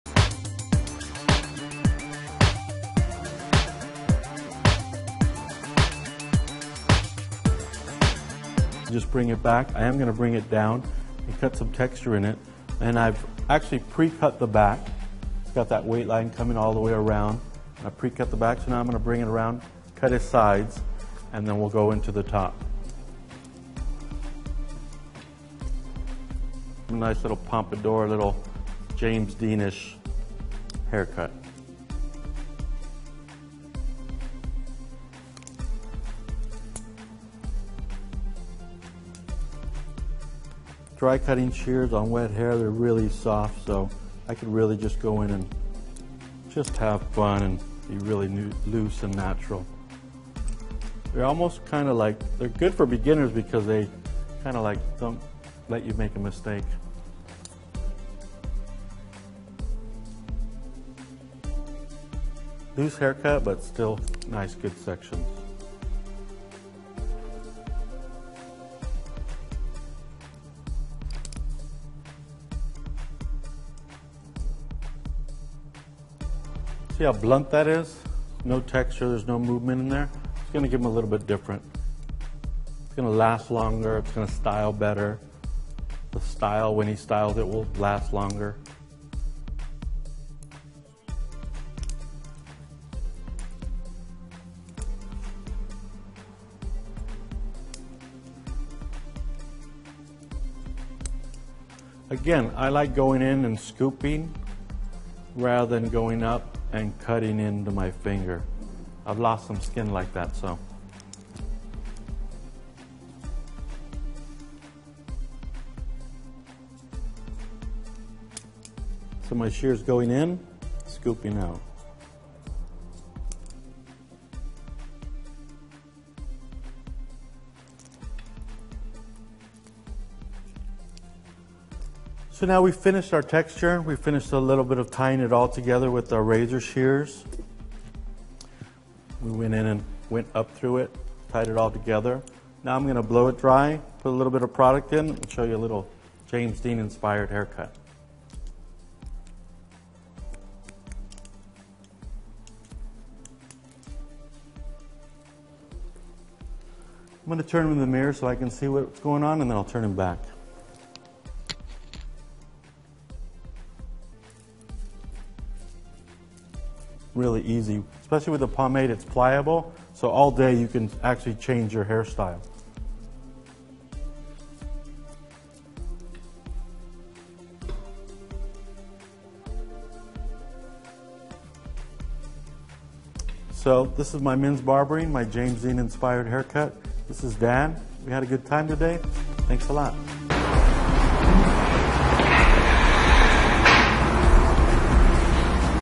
Men haircuts: Classic pompadour textured haircut, James Dean inspired
Hey, this is a classic textured haircut, James Dean inspired.